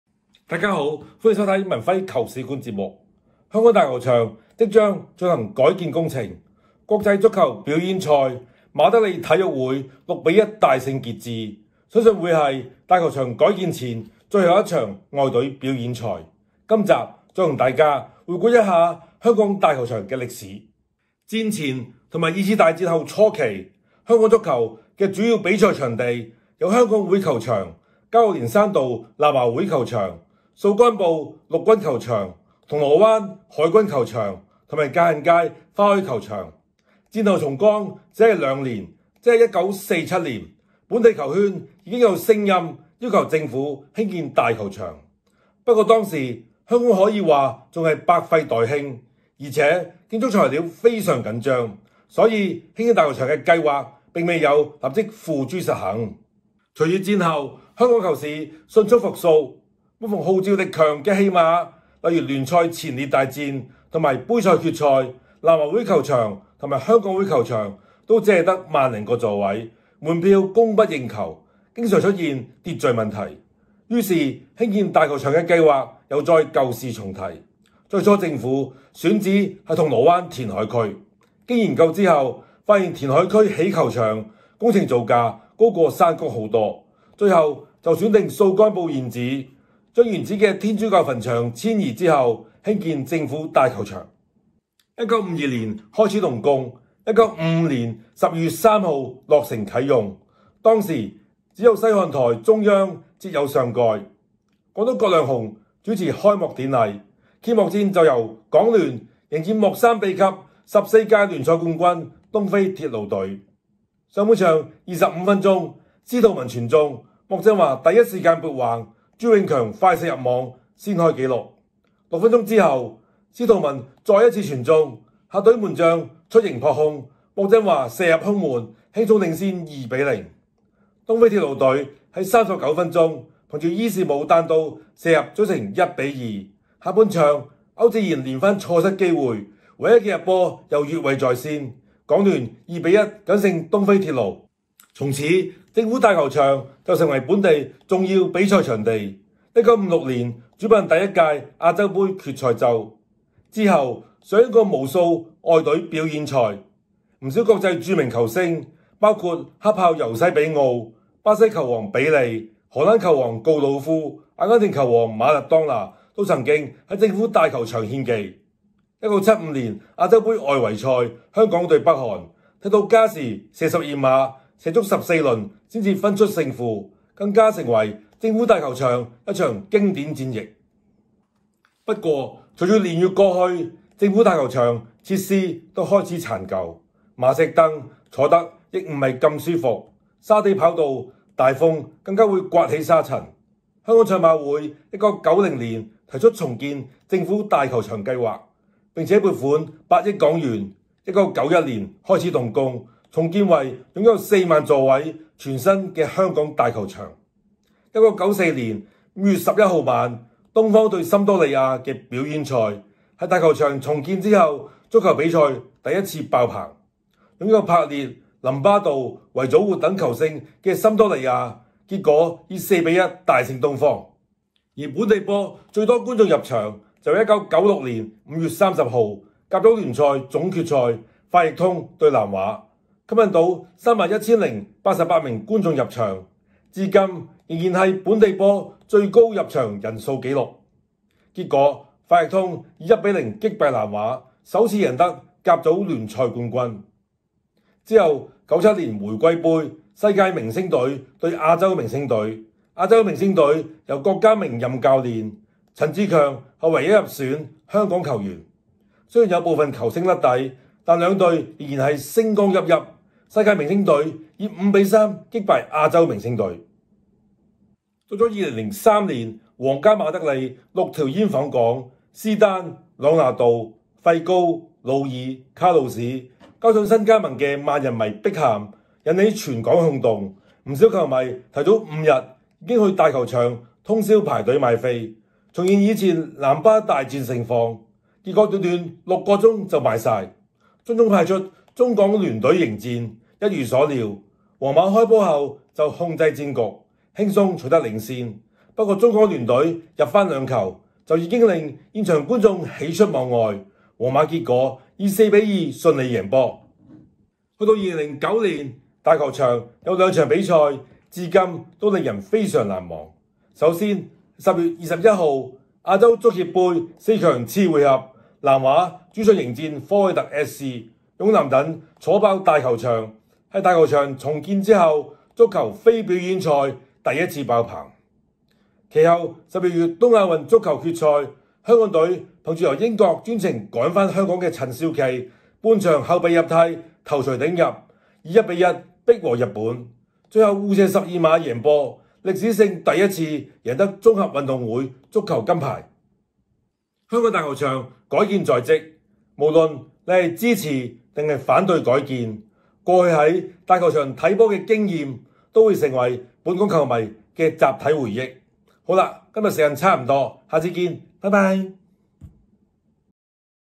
文輝球史館 第五十四集 : 香港大球場歷史
香港足球史學會創會秘書賴文輝，係《文輝球史館》節目同大家講故仔，分享香港足球歷史話題